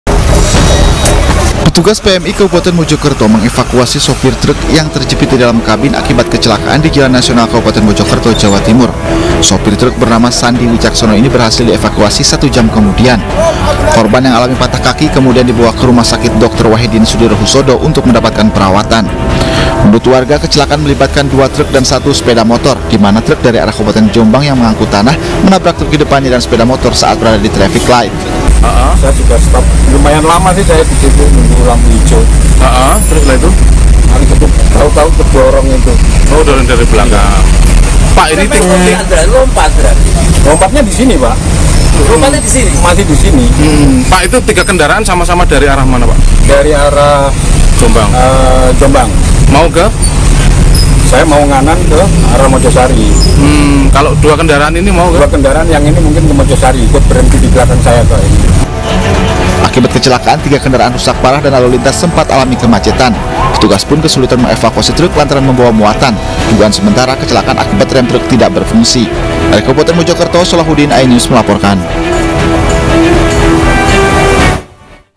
GEGER! Truk Tabrakan di Mojokerto, Sopir Alami Patah Kaki
Evakuasi sopir truk terjepit kabin dalam kecelakaan di Jalan Raya Nasional Kenanten, Kecamatan Puri, Kabupaten Mojokerto berlangsung dramatis.